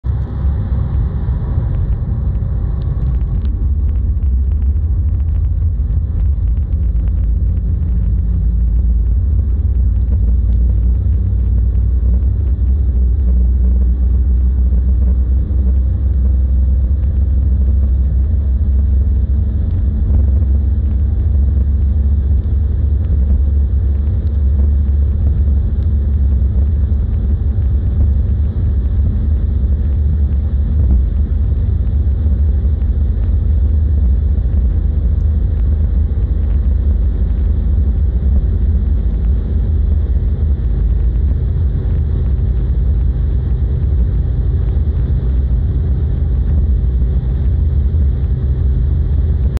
2010 Ford Focus 1.6 TDCI 100-190
Acceleration from 100 to 190 kph on german Autobahn. Forc Focus 1.6 TDCI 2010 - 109 HP